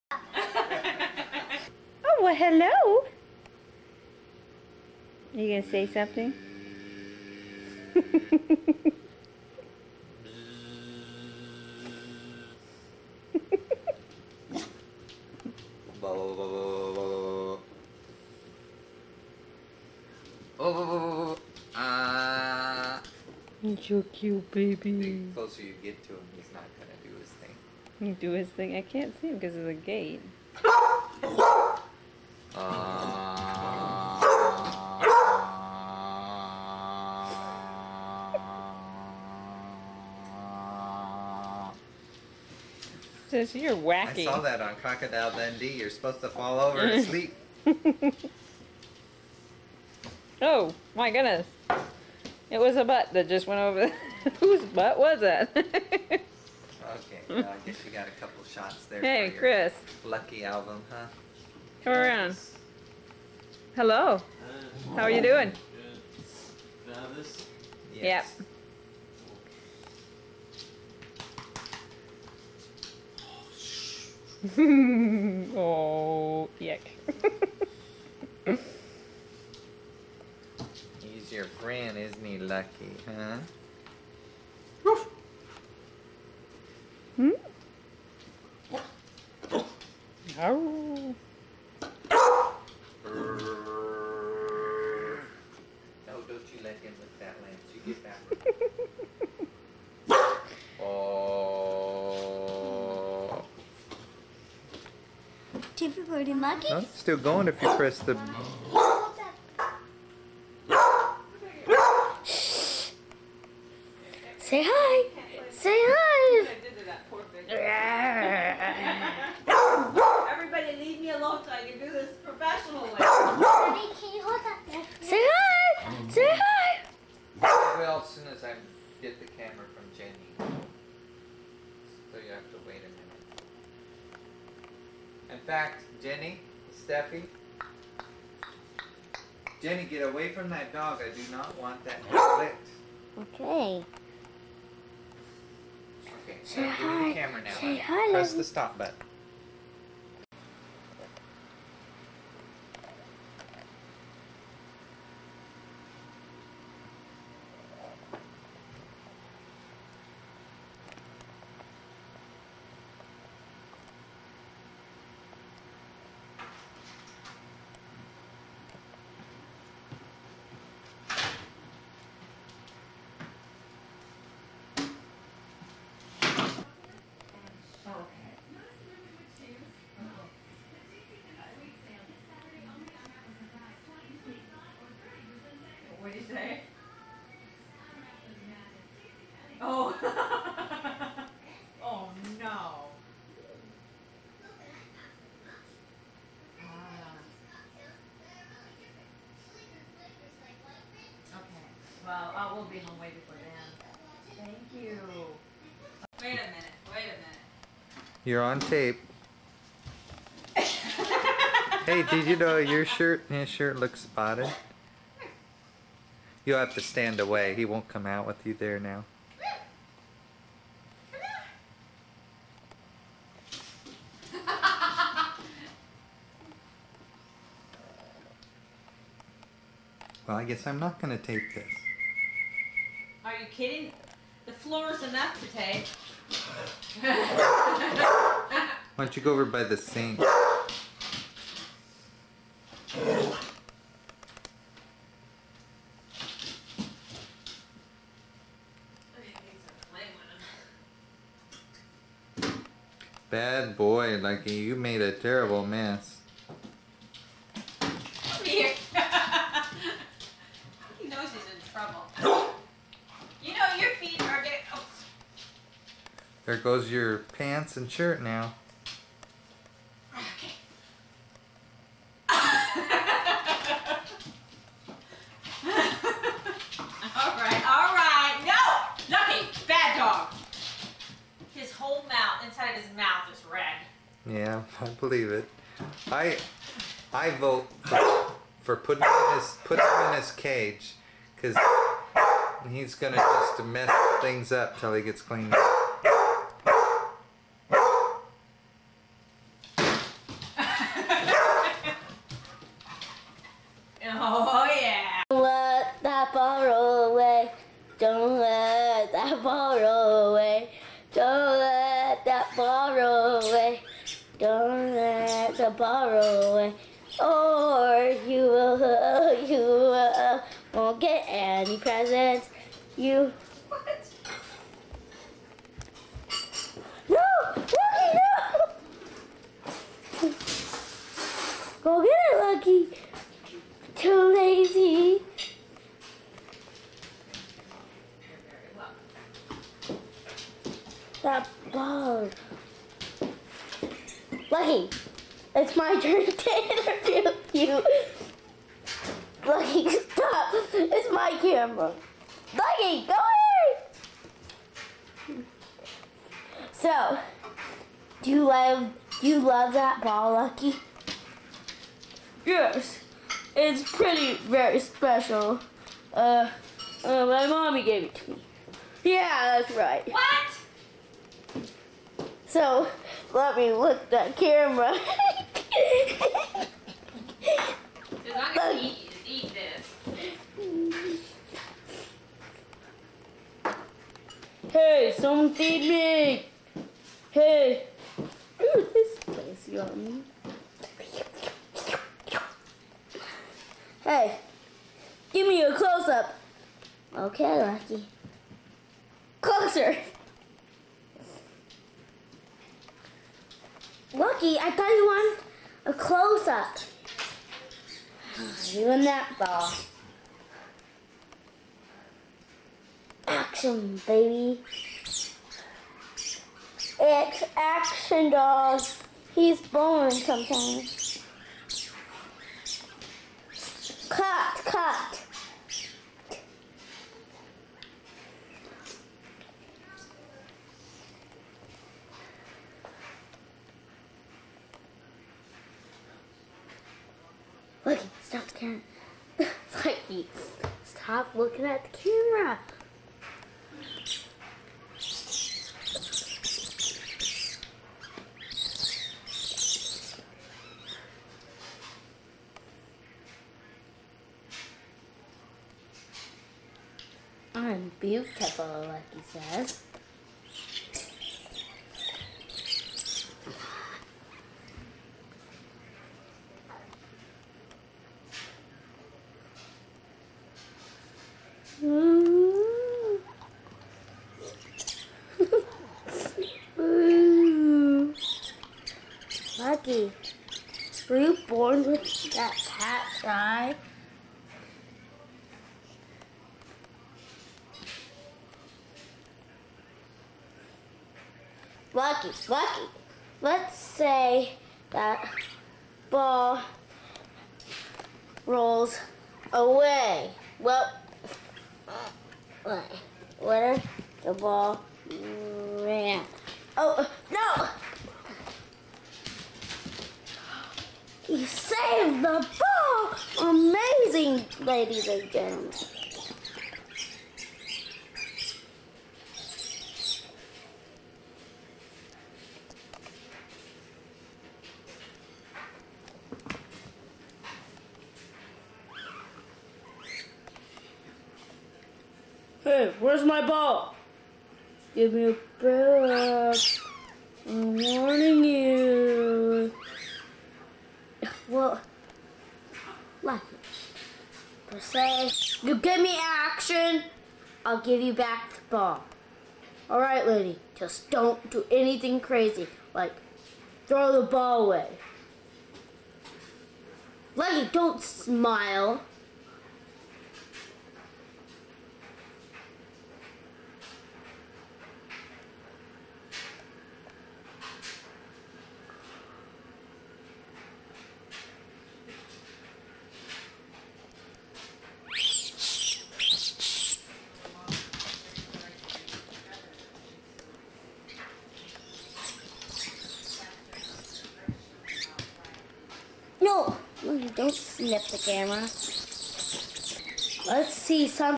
Approx 2000 Pictures Of Lucky And Our House In 4K Ultra HD
This is a home video of pictures of our dog named Lucky. The video was done in approximately the year 2000 in Olympia Washington.  It has been converted to 4K Ultra HD streaming media so it can be viewed on Smart Tvs, Smart Phones, and computers.